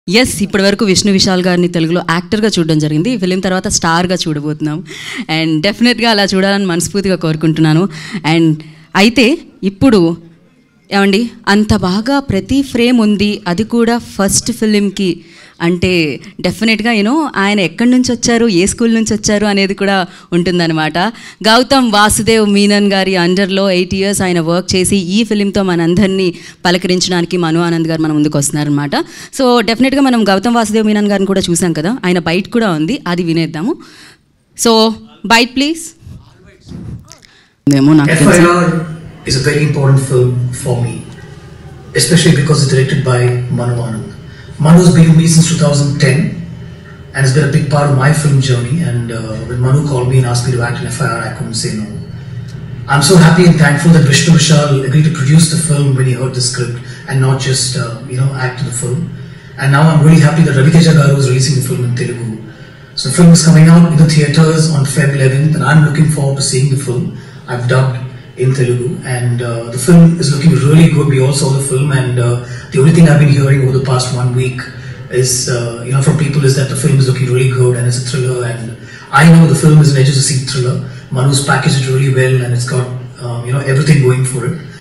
Jwala Gutta And Vishnu Vishal CUTE Moments At FIR Movie Pre Release Event | Daily Culture
Jwala Gutta And Vishnu Vishal CUTE Moments At FIR Movie Pre Release Event